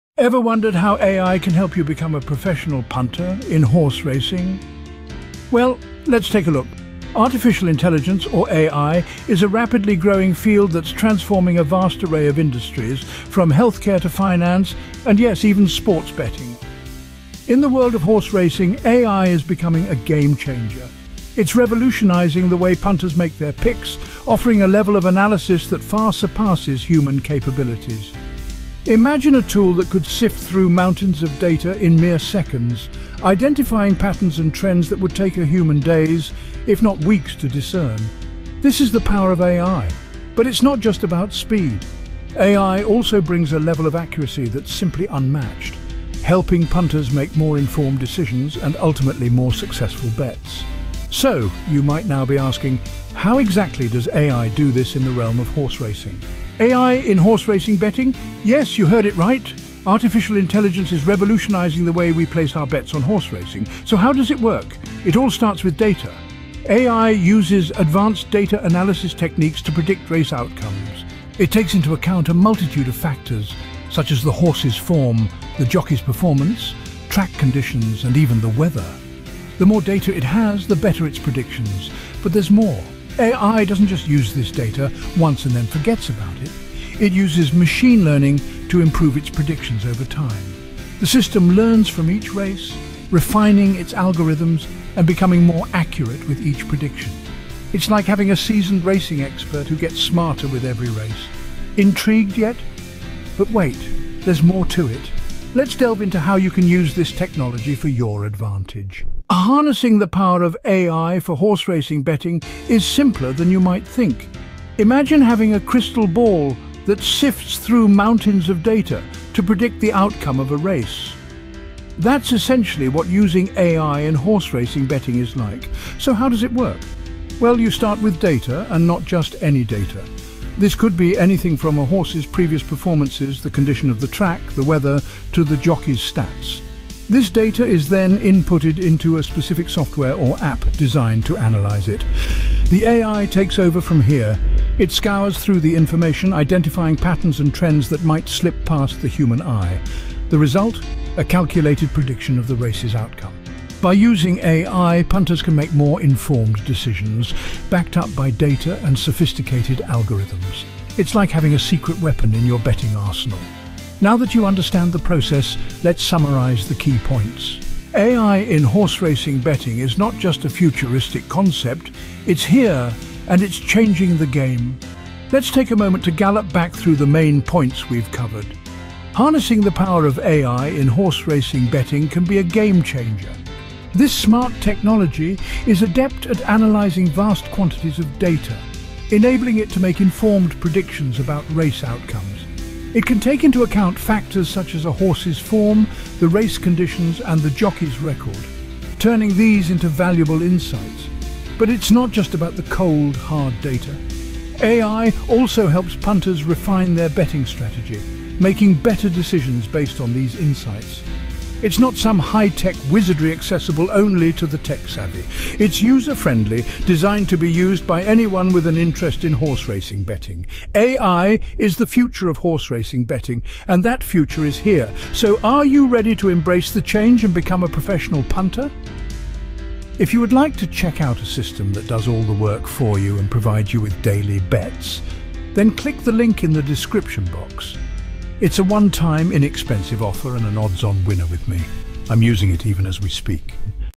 Unveiling the Secret to AI in Horse Racing
In this video, we uncover the secrets to becoming a pro punter at horse racing using the power of Artificial Intelligence (AI). Dive into the world of horse racing betting as we guide you through the process of harnessing AI algorithms to analyze data, predict outcomes, and make informed betting decisions. Don't miss out on this opportunity to revolutionize your approach to horse racing betting with AI!
Click the link ( or copy and paste) below to check out a done for you betting system using AI.